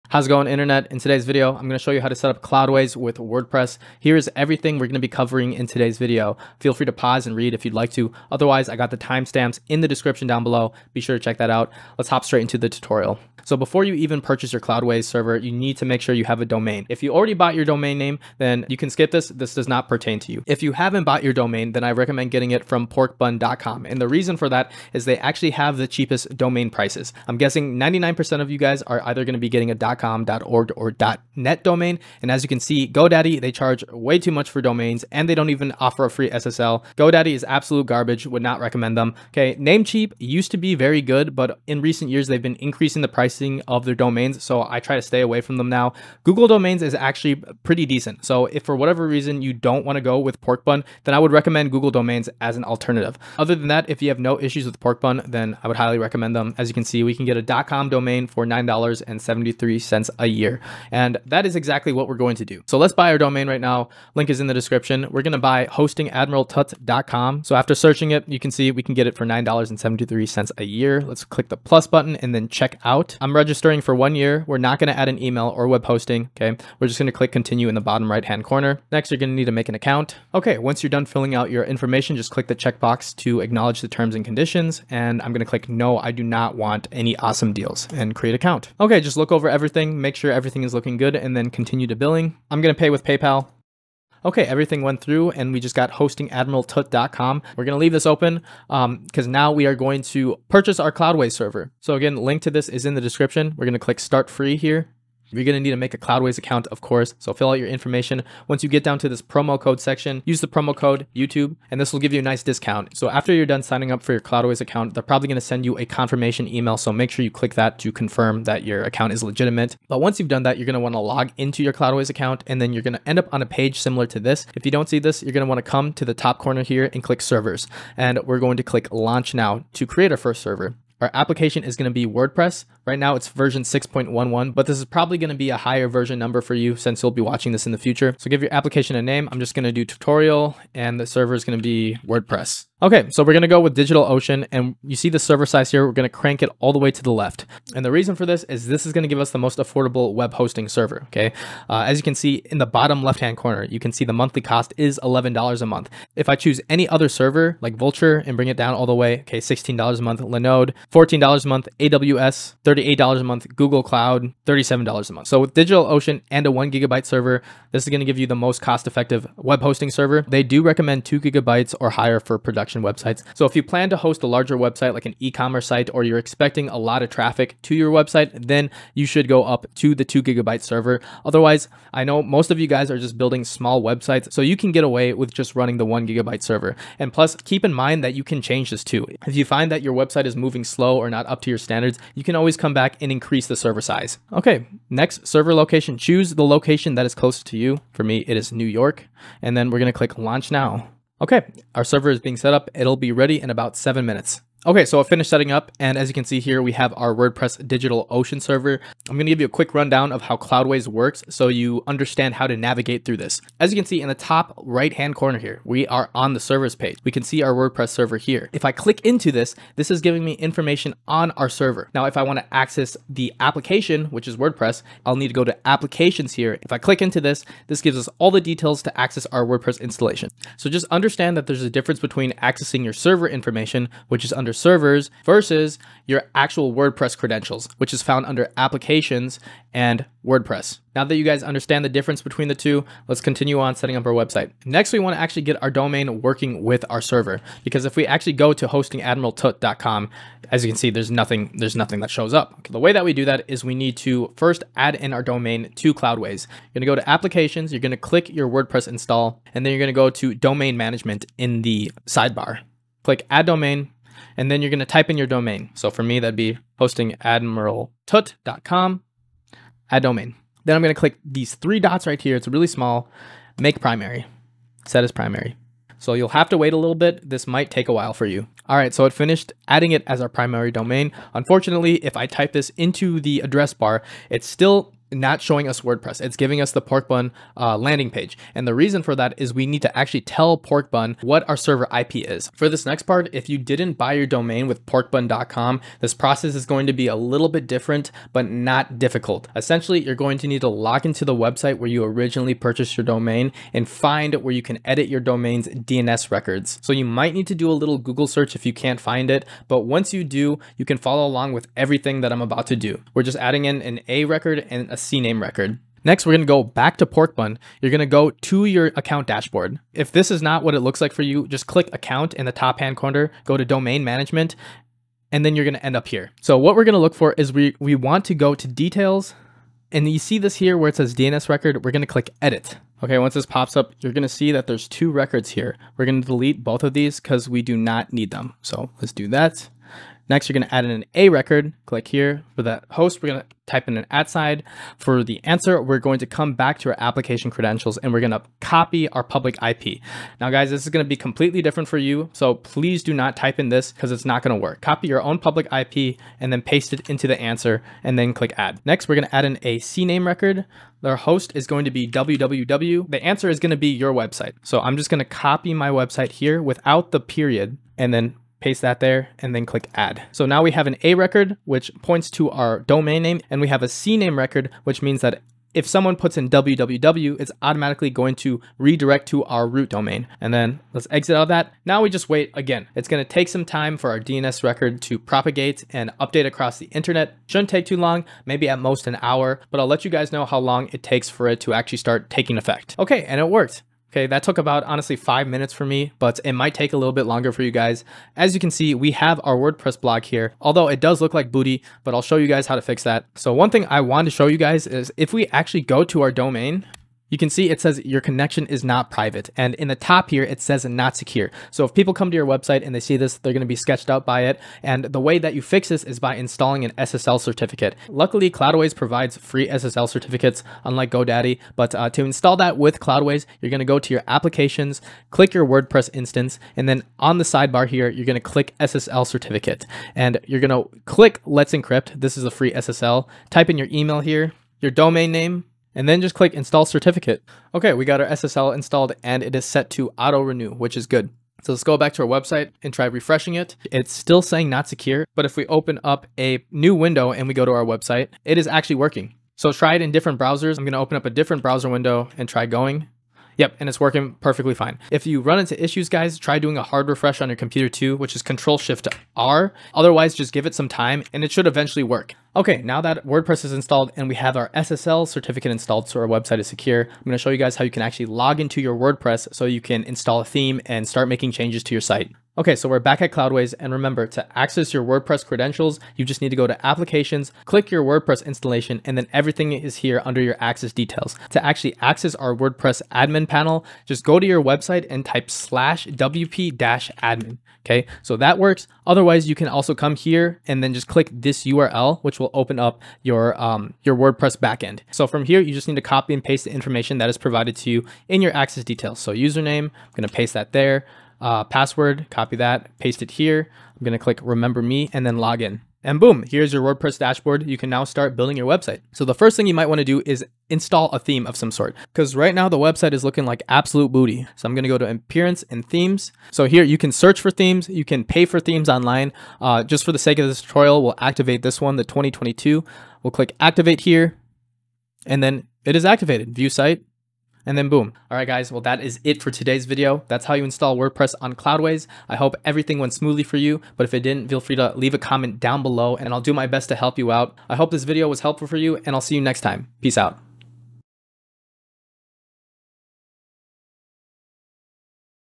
How to setup WordPress with Cloudways Web Hosting
In this video I show you guys how to set up Wordpress with Cloudways web hosting. The method I show you in this video is probably the most cost effective way to start your website since we're saving money purchasing our domain through a 3rd party registrar (Porkbun) and we're using the cheapest Cloudways server ($11/mo).

I do think Cloudways is the best web hosting provider available at the moment when you take into account all the services Cloudways provides you with along with the price and performance of your server.

TIMESTAMPS
0:00 - Intro
0:13 - Domain registrar recommendations
1:01 - Purchasing Domain
1:39 - Sign up for Cloudways
2:07 - Installing WordPress with Cloudways
3:28 - Simple Cloudways tutorial
4:04 - Setting up domain with Cloudways
4:48 - Setting up Cloudways DNS records
6:45 - Setting up Let's Encrypt with Cloudways
7:42 - Logging Into WordPress
8:29 - Activating a theme